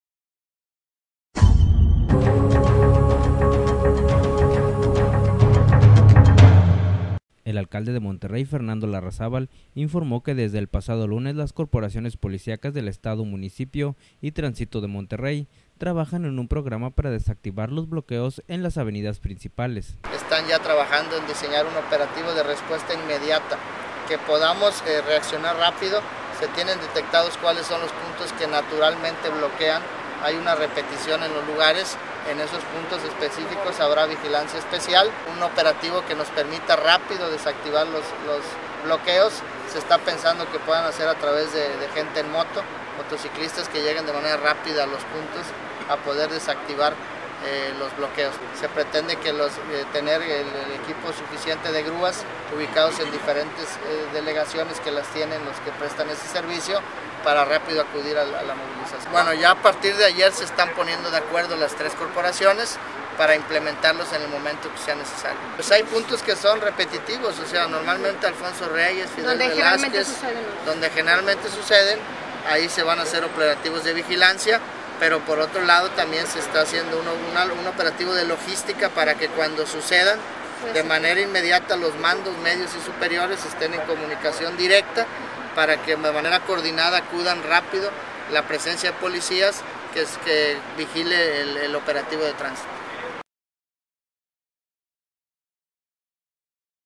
ACUERDAN OPERATIVO ANTIBLOQUEO.wmv
Las autoridades se preparan para tratar de solucionar de inmediato los bloqueos que se puedan presentar por las calles de la ciudad.